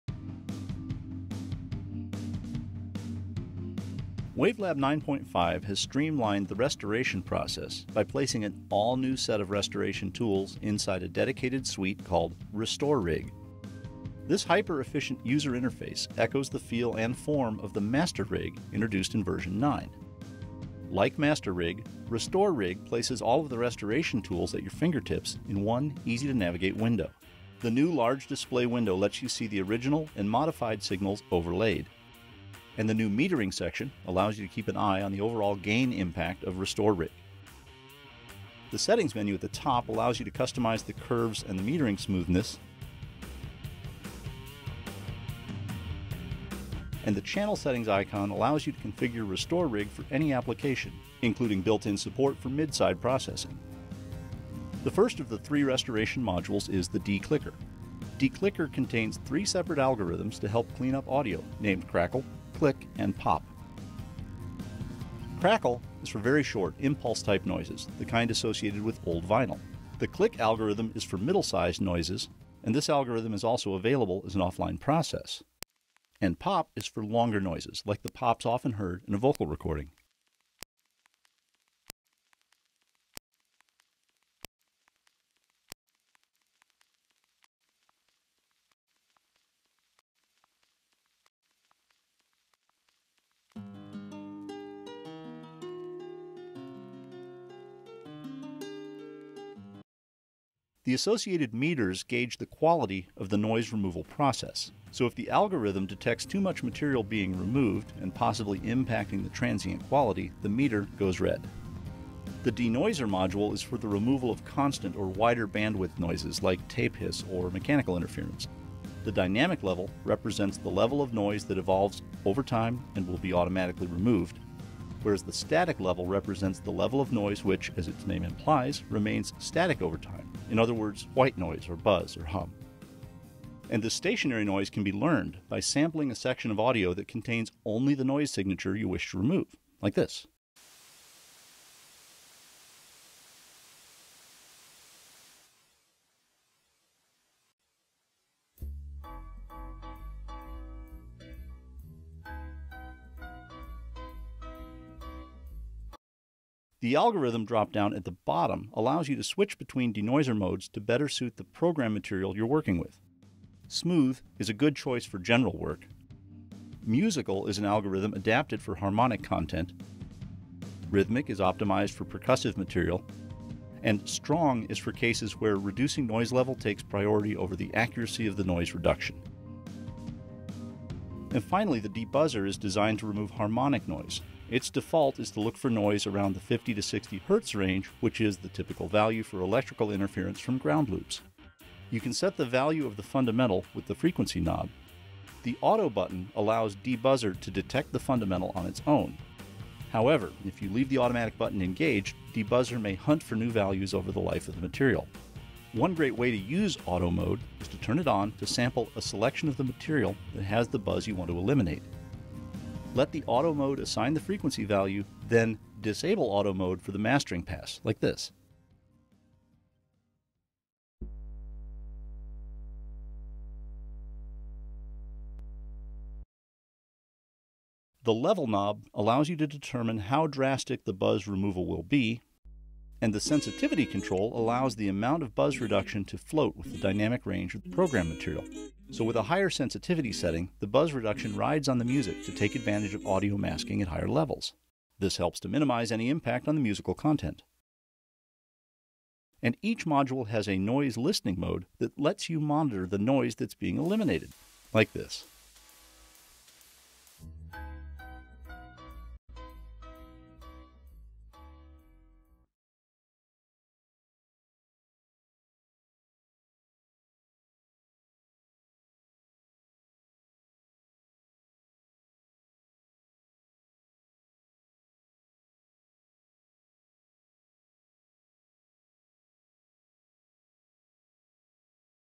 The RestoreRig Plug-in Suite | New Features in WaveLab Pro 9.5
RestoreRig is the new restoration plug-in suite in WaveLab Pro 9.5, comprising a DeNoiser, DeClicker and a DeBuzzer. Using this plug-in is pretty much self-explanatory, but in this video we’ll take a detailed look at RestoreRig’s powerful features and settings and listen in on some audible examples of its extraordinary quality.